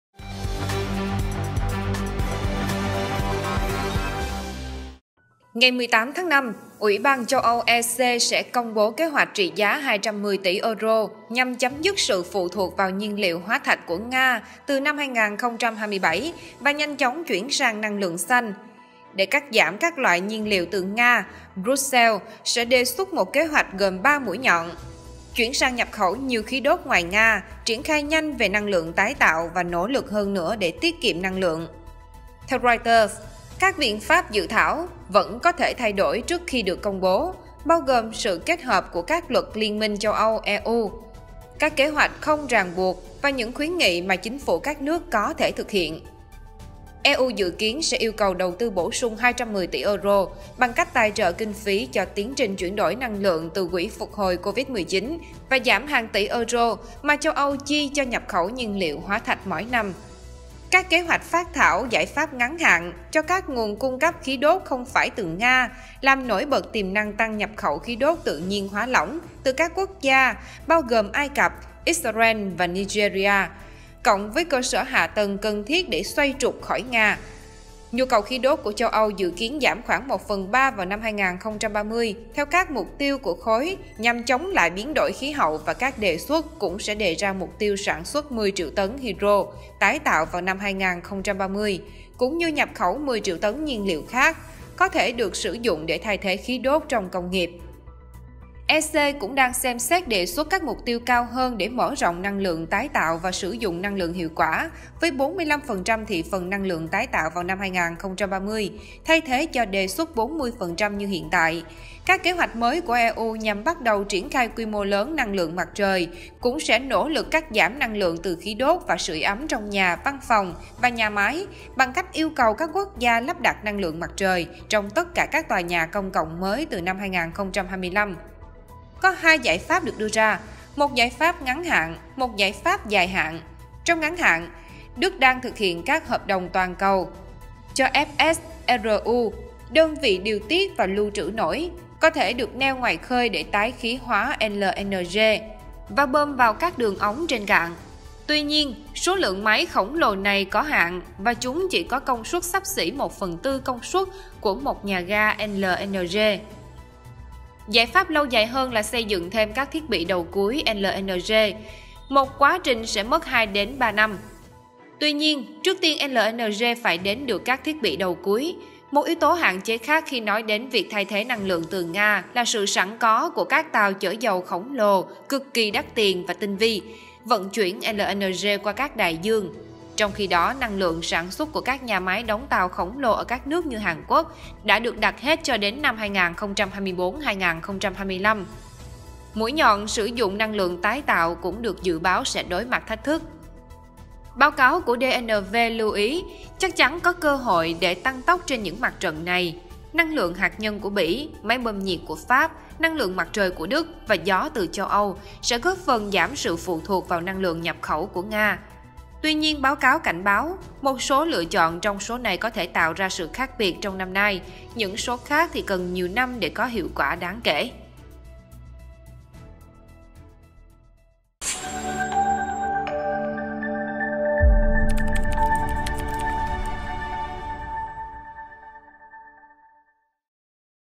EU công bố kế hoạch 210 tỷ euro, với 3 mũi nhọn thoát khỏi năng lượng Nga
Ngày 18-5, Ủy ban châu Âu (EC) sẽ công bố kế hoạch trị giá 210 tỷ euro nhằm chấm dứt sự phụ thuộc vào nhiên liệu hóa thạch của Nga từ năm 2027, và nhanh chóng chuyển sang năng lượng xanh.